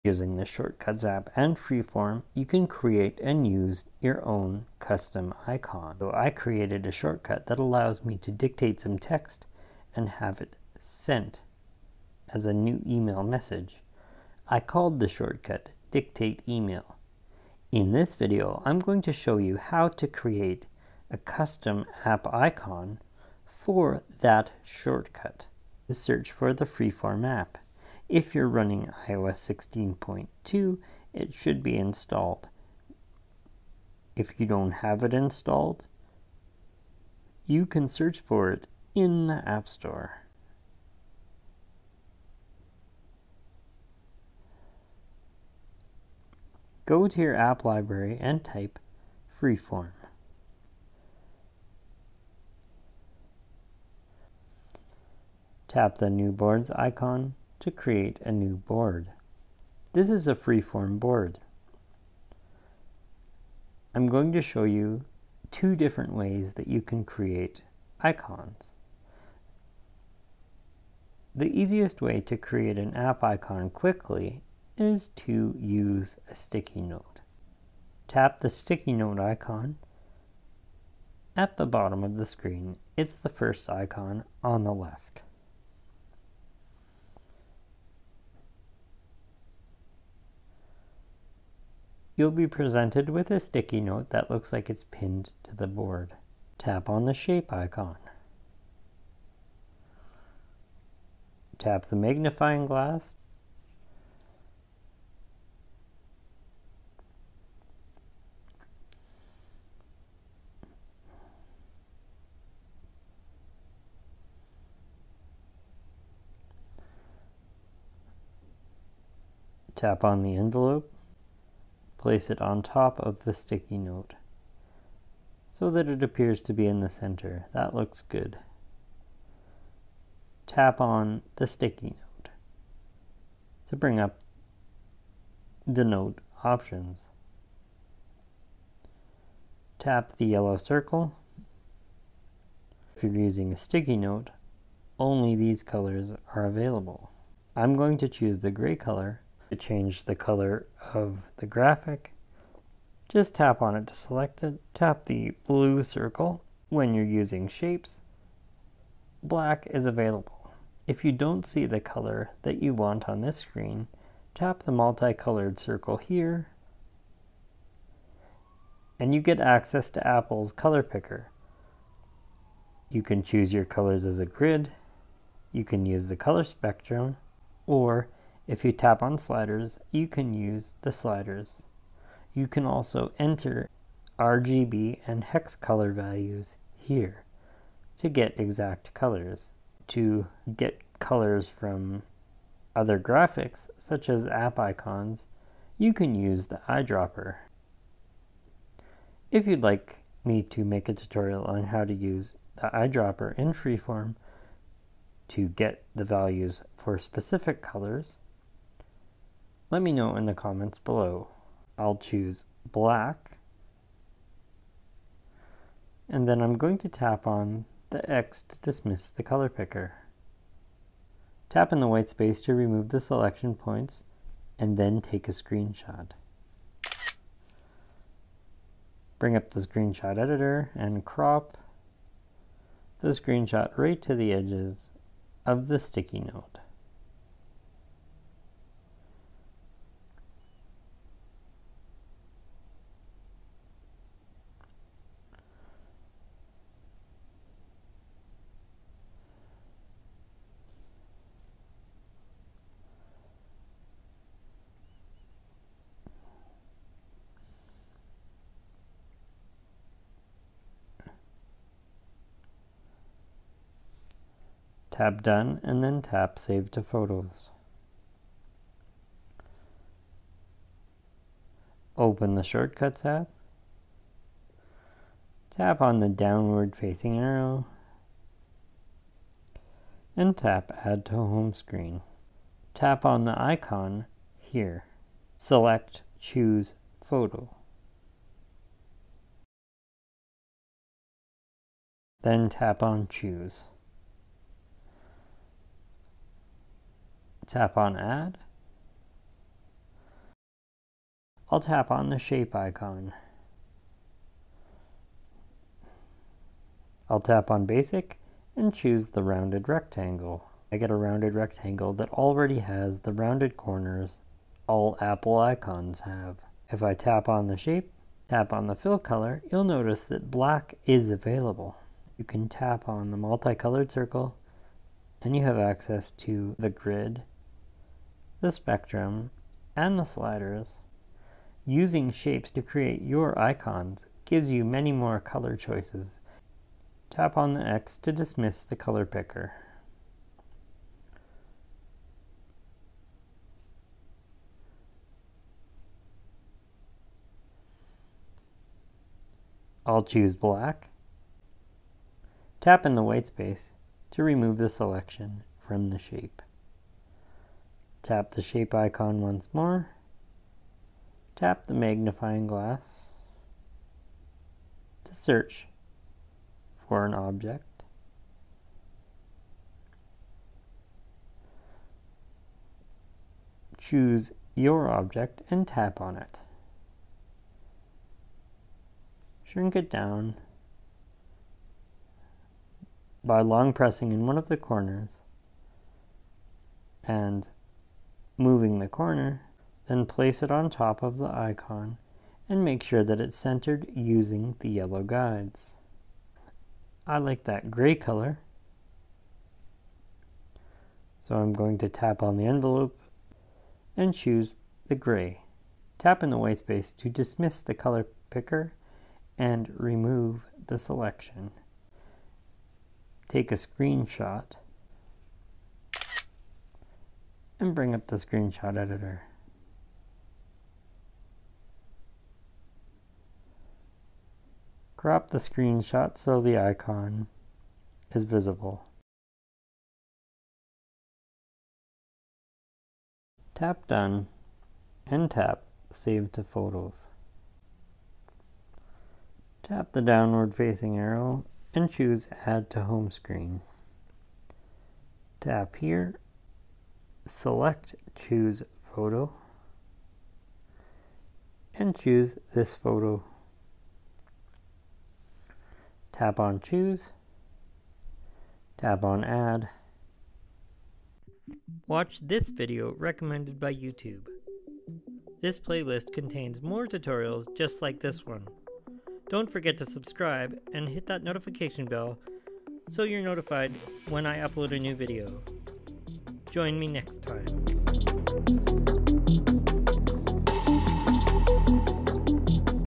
How To Create Icons In Freeform
You can create your own custom app icons with the shortcuts app and FreeForm
Here’s how:
0:00 How to create icons in Freeform
1:01 How to create i icons Using Sticky Notes
5:00  How to create icons with shapes
7:40 Video End